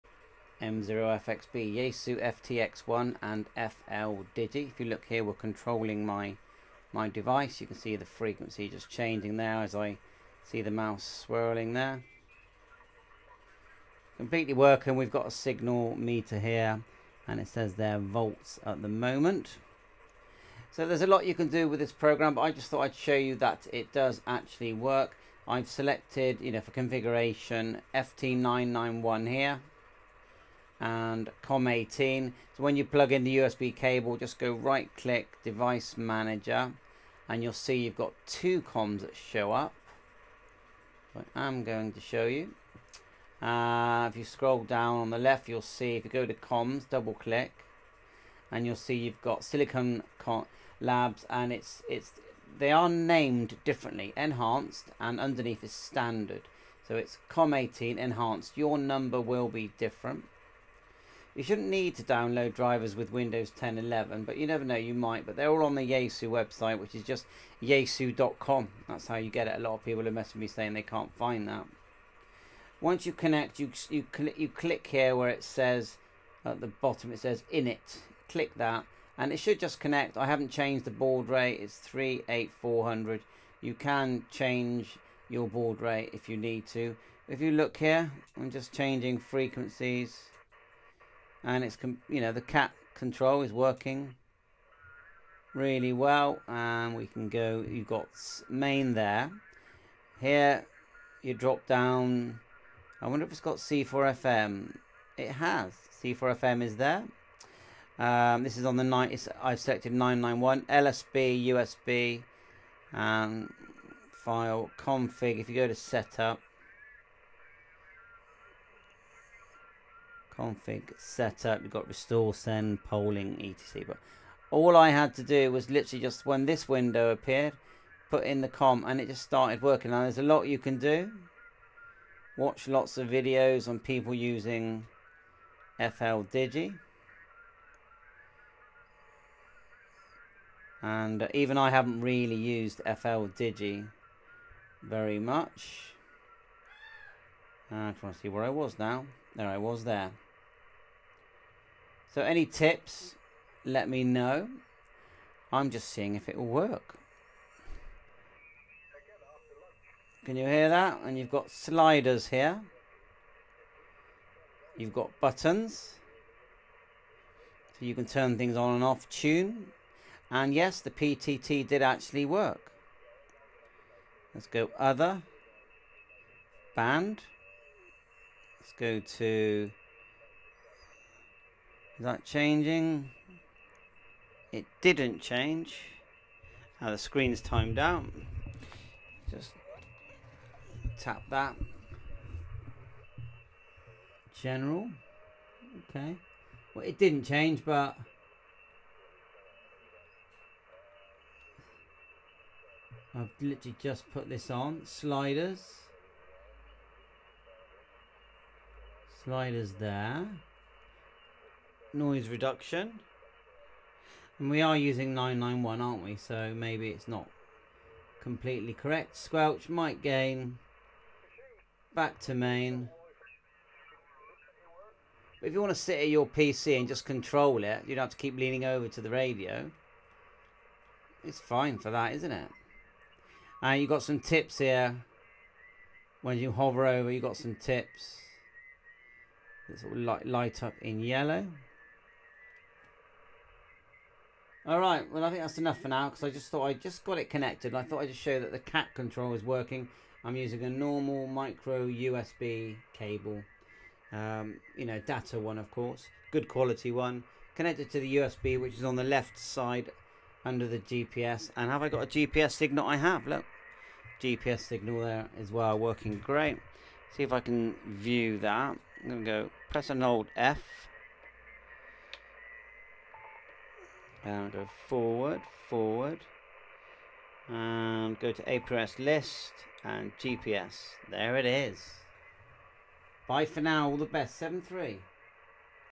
Fldigi - Yaesu FTX-1 , Cat Control
DOWNLOAD FLDIGI

Yaesu FTX-1
"SDX" or Super-DX function, is designed to enhance weak signal reception by increasing the receiver's sensitivity. This feature is often activated via a dedicated SDX button and works by providing preamplification and noise reduction, allowing users to pick up signals that might otherwise be missed, especially in crowded or noisy environments
"averaging" refers to a feature that reduces noise in a received signal by averaging multiple readings over a period of time. This can be particularly useful in noisy environments or when receiving weak signals, as it helps to improve the clarity and readability of the received information. Additionally, iCOM radios may also use averaging in other contexts, such as calculating the average power output or other signal characteristics.